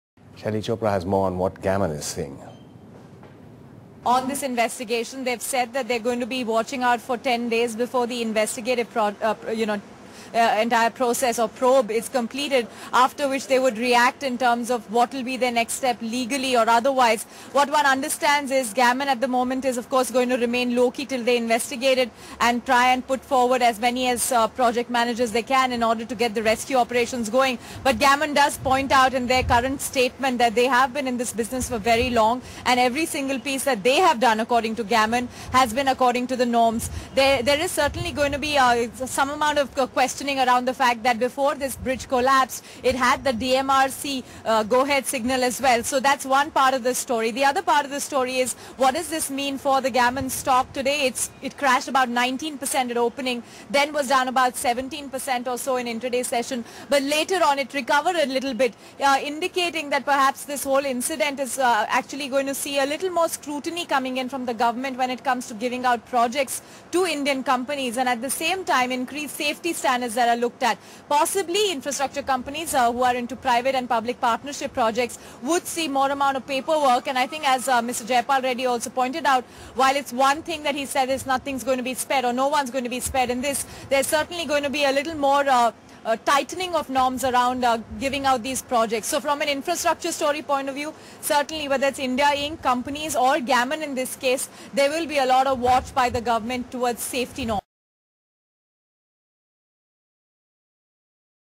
Delhi Metro mishap: What next for Gammon?
Shares of Gammon India fell today following an accident at a Delhi Metro site.